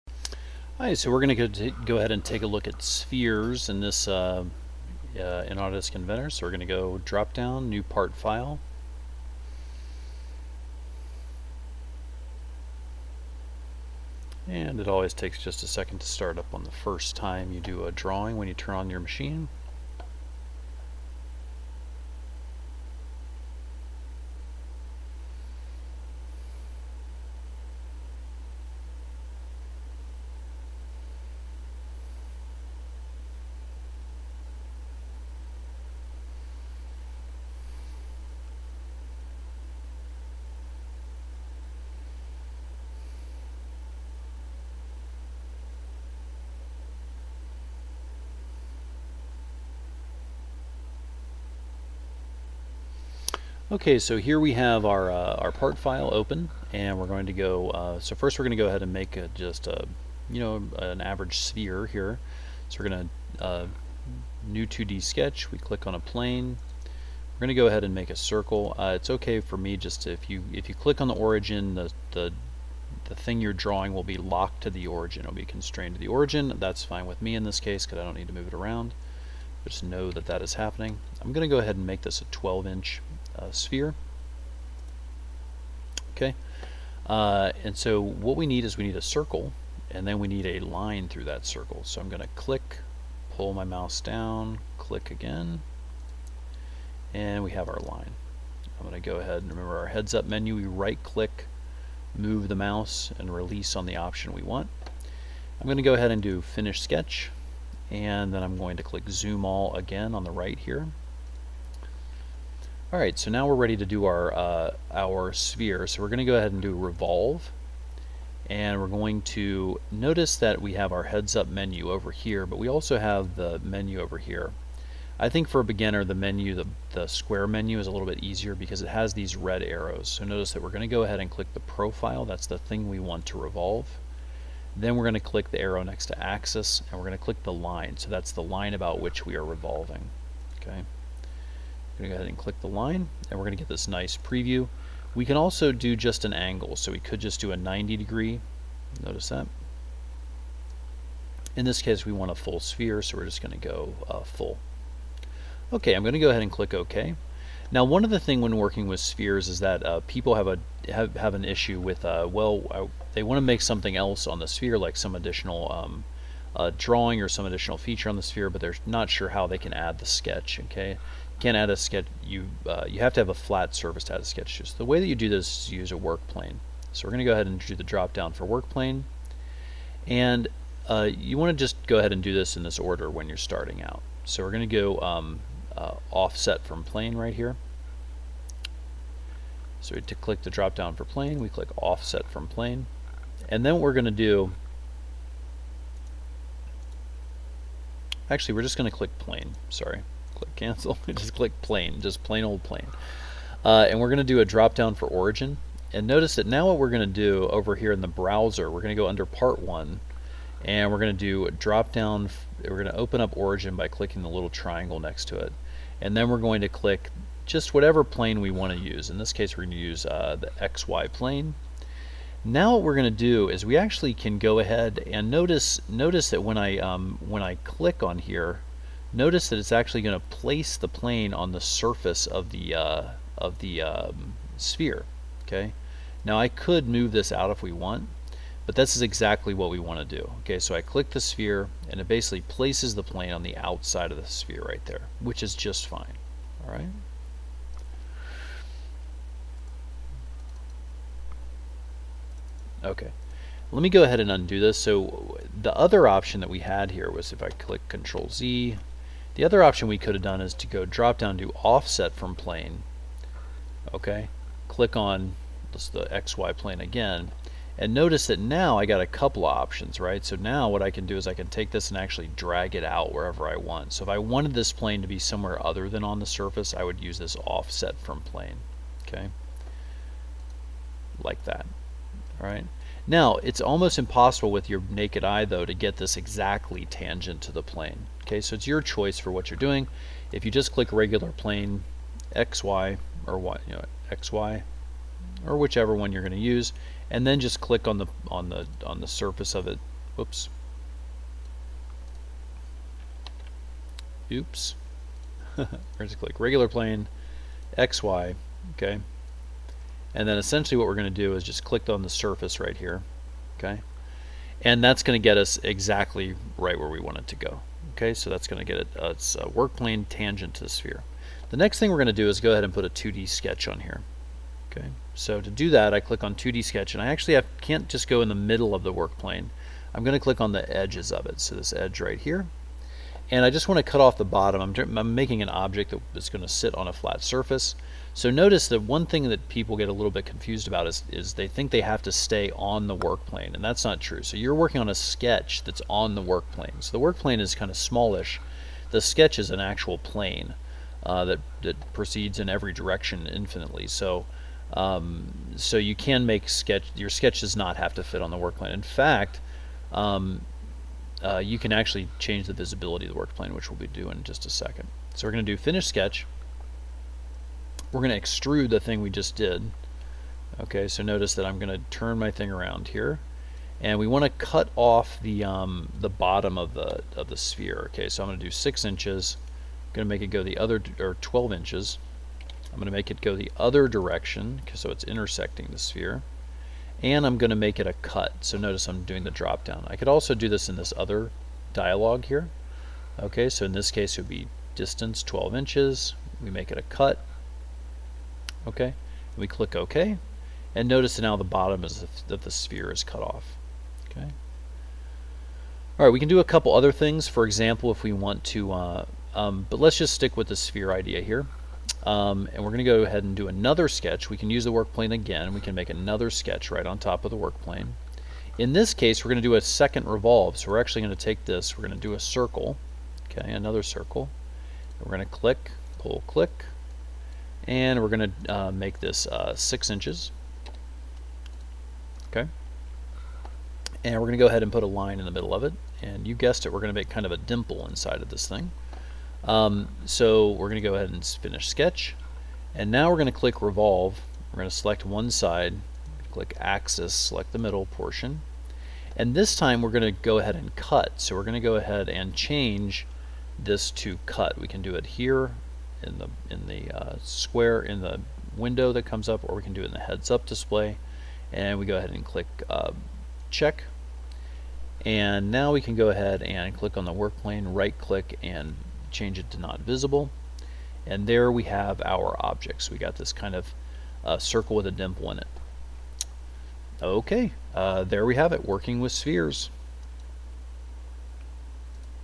Autodesk Inventor 2017 : 11 : Creating a Sphere
Tutorial lesson on how to create and work with spheres in autodesk inventor